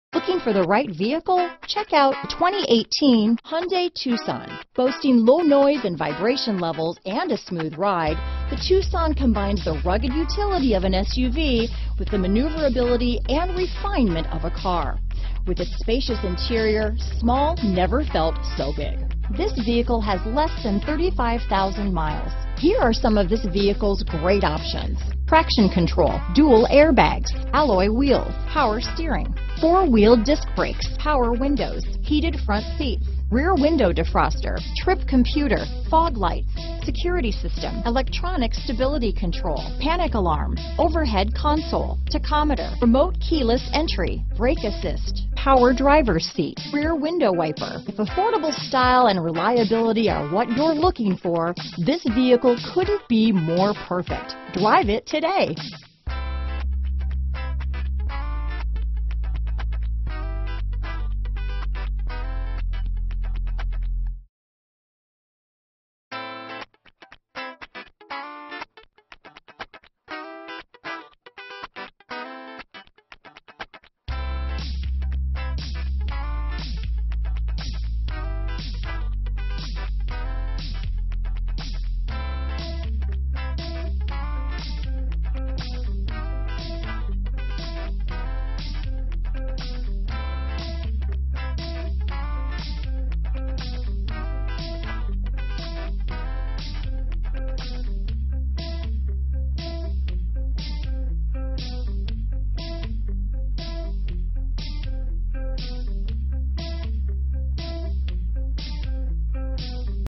2018 Hyundai Tucson Carson City, Lake Tahoe, NV CC1559
Caribbean Blue [LISTING TYPE] 2018 Hyundai Tucson available in Carson City, Nevada at Carson Car Center. Servicing the Lake Tahoe, NV area.

WE HAVE A NEW OWNER AND WANT TO TAKE CARE OF ALL OF NORTHERN NEVADA. AWD. 2018 Hyundai Tucson SEL CARFAX One-Owner. Clean CARFAX.brbrSEL